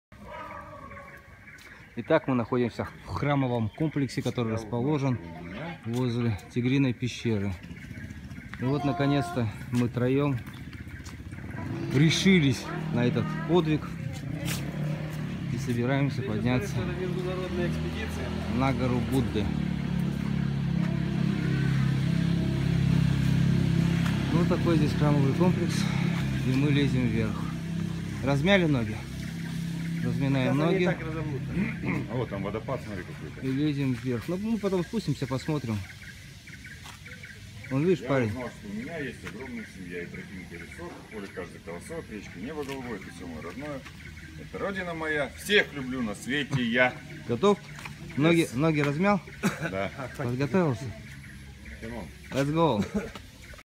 Автопутешествие по Таю
Тигровая пещера, храм на вершине горы. Нам предстоит забраться на верх и обозреть вид сверху на Краби.